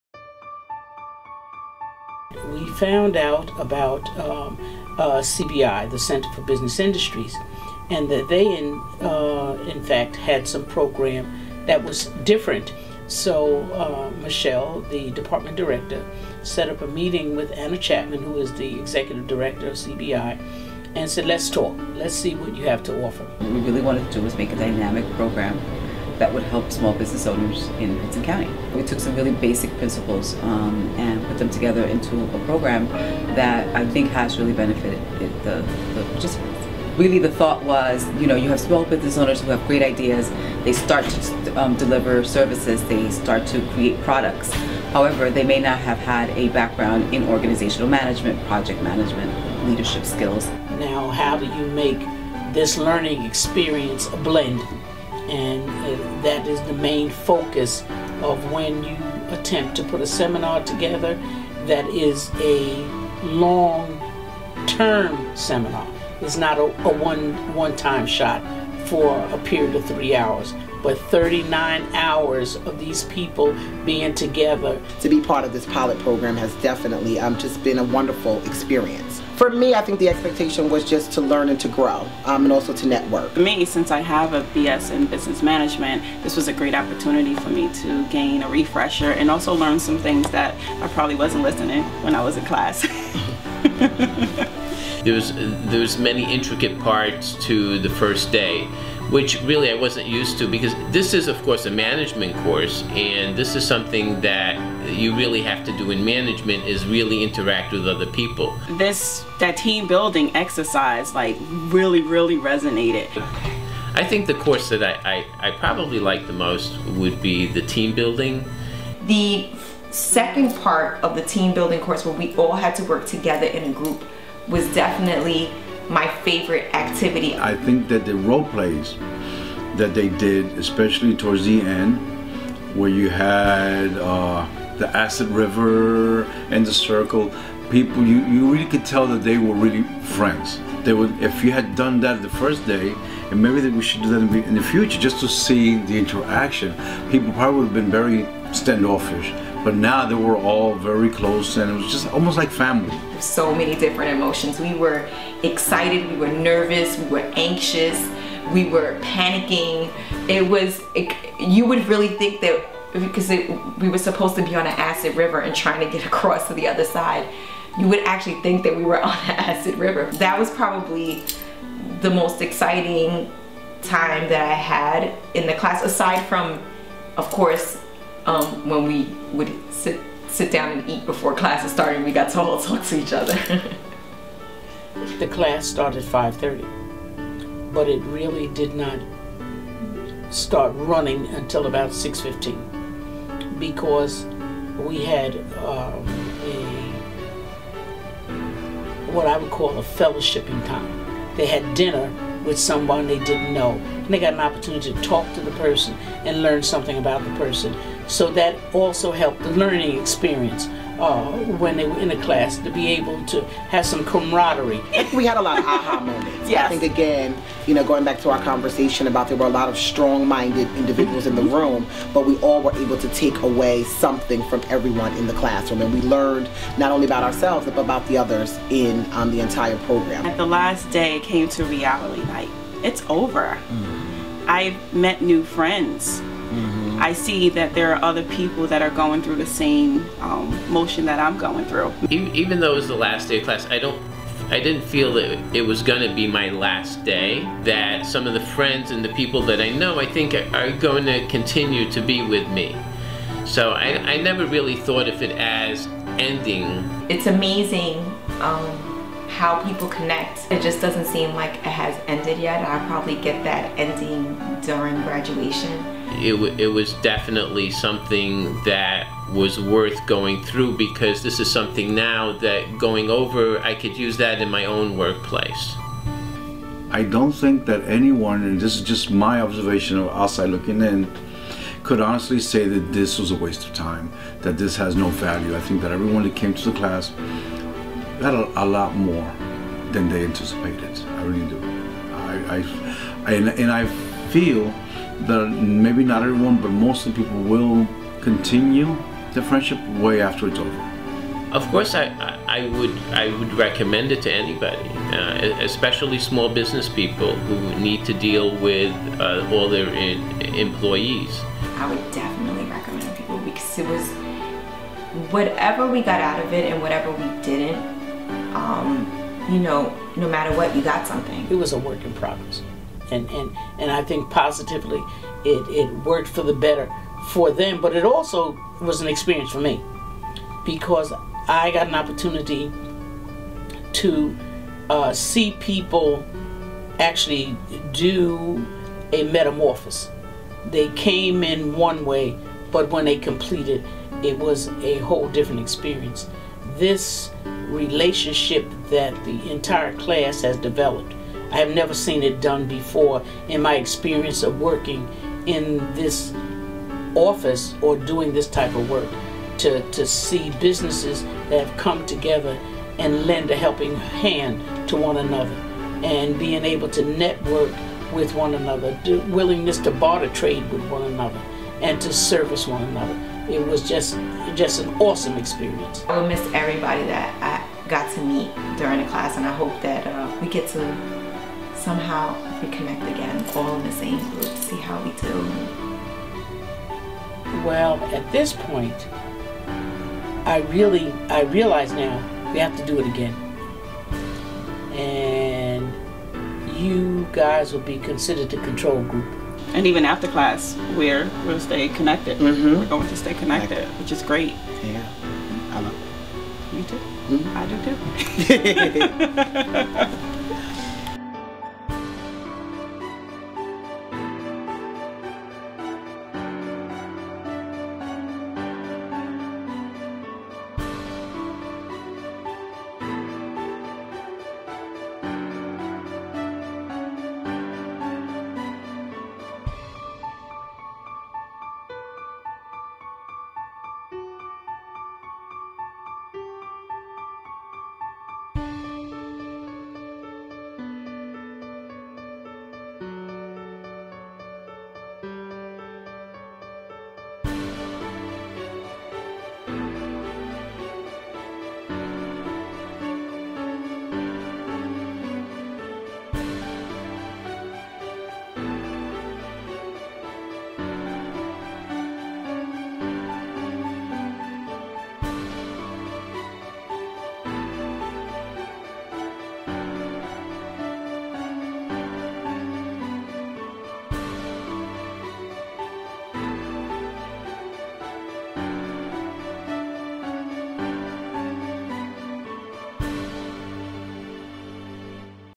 Hudson County Business Management Training Program
This program was created for small business owners to learn Management skills to use in their businesses.